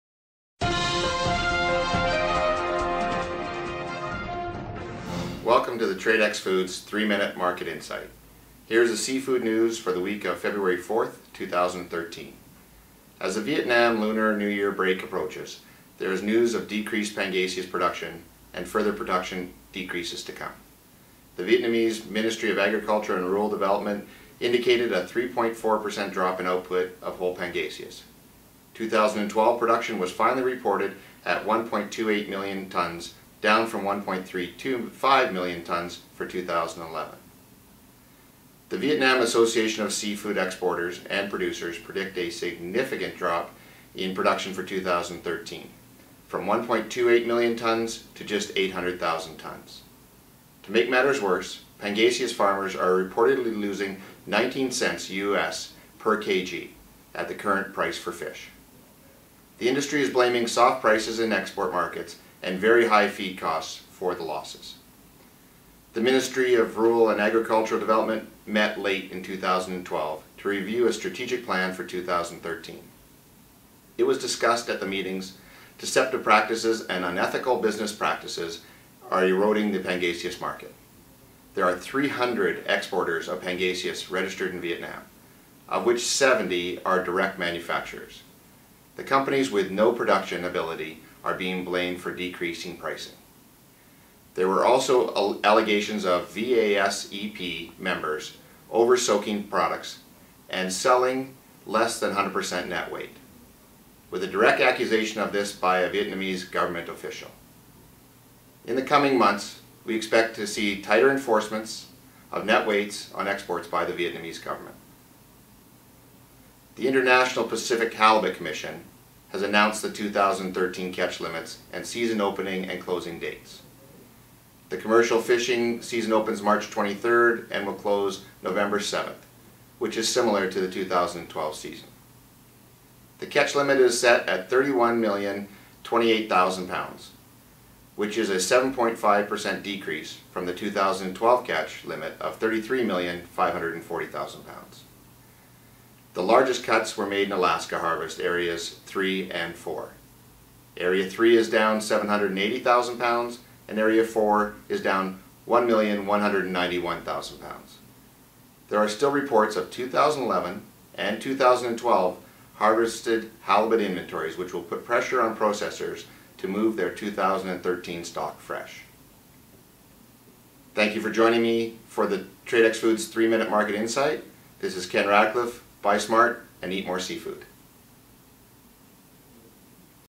3-Minute Market Insight EP118 - Halibut, Vietnam Seafood Production as Lunar New Year Approaches
This week in the Tradex Foods 3-Minute Market Insight, President & CEO Rob Reierson discusses...
- Vietnamese Ministry of Agriculture & Rural Development indicated a 3.4% drop in output of Whole Pangasius
- Vietnam Association of Seafood Exporters & Producers Predict a Significant Drop in Production for 2013 from 1.28 Million Tons to just 800,000 Tons
- Pangasius Farmers are Reportedly Losing the Vietnamese Dong Equivalent $0.19 USD per kg. at the Current Price for Fish
- Pacific Halibut Commission has announced the 2013 Catch Limits